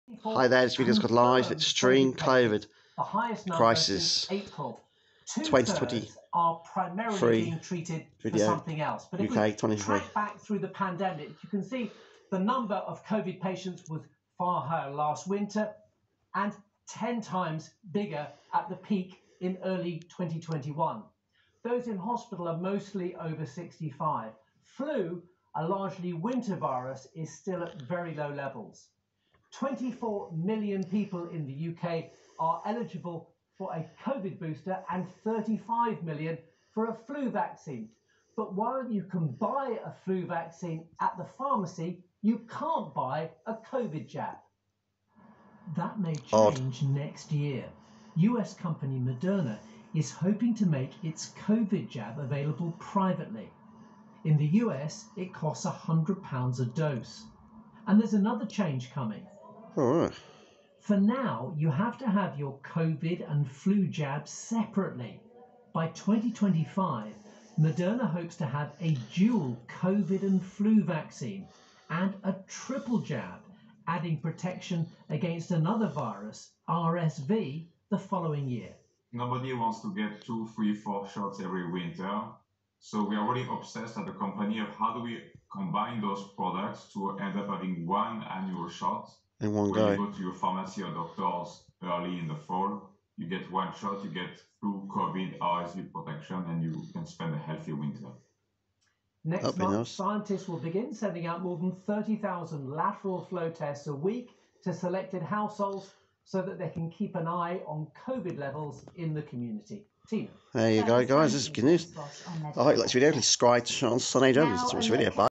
LIVE COVID CRISIS VIDEO UK2023.👩🏼‍🔬🦠⚠️🚨👺👍
TALK ABOUT YOU BEEN ABLE TO BUY COVID BOOSTER NEXT YEAR PRIVATELY. AND TALK ABOUT COVID CASES GOING BACK UP ACROSS UK ESPECIALLY IN THE SOUTH EAST AND SOUTHERN ENGLAND REGENTS NO JOKE. 👩🏼‍🔬🦠⚠️🚨👺👍